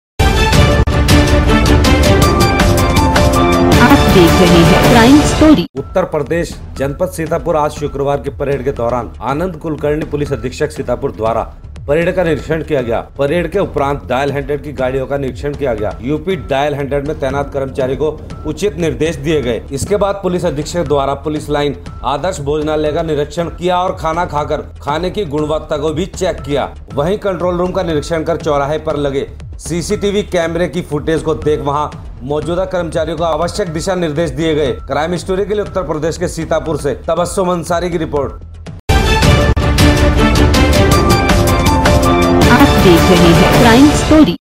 परेड के दौरान पुलिस अधीक्षक द्वारा किया गया निरीक्षण
( क्राइम स्टोरी के लिए उत्तर प्रदेश के सीतापुर से संवाददाता तबस्सुम अंसारी की रिपोर्ट I )